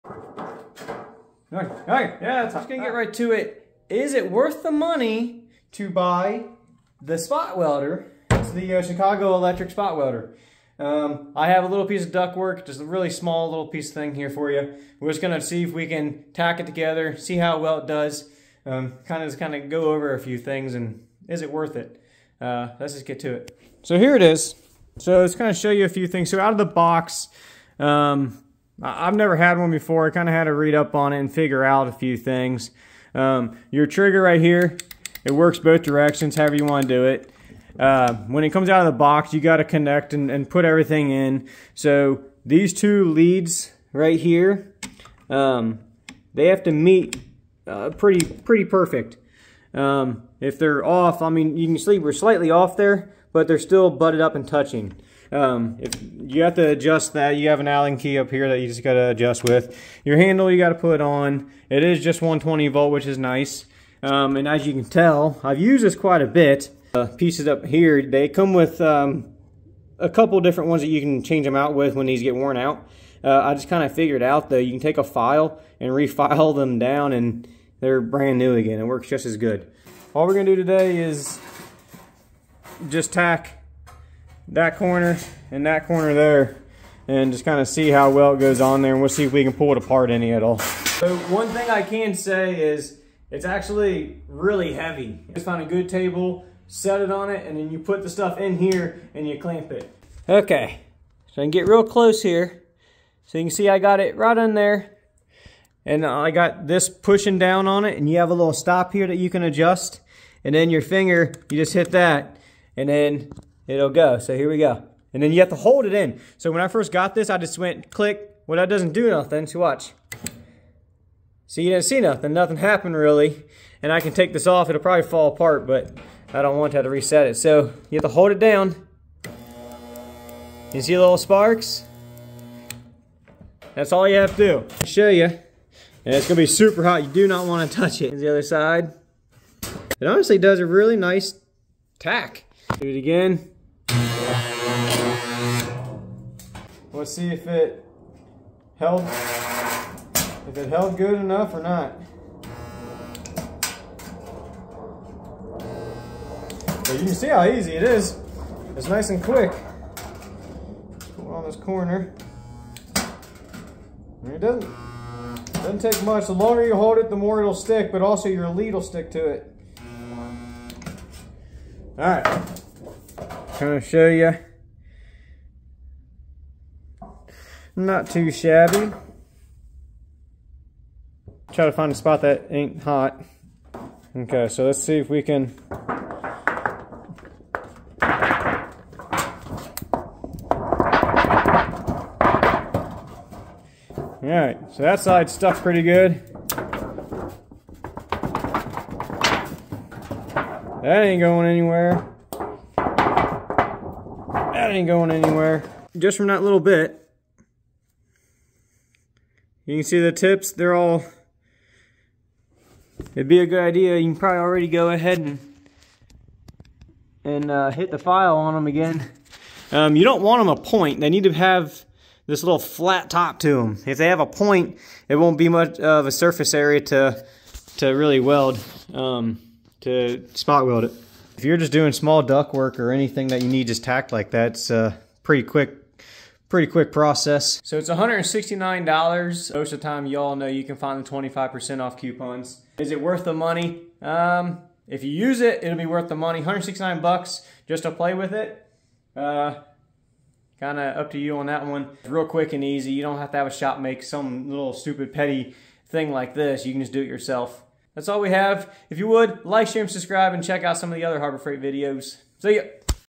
Worst Harbor Freight Purchase Yet? Harbor Freight Spot Welder
Review over the Harbor Freight spot welder. Is it worth the money? Watch this before buying!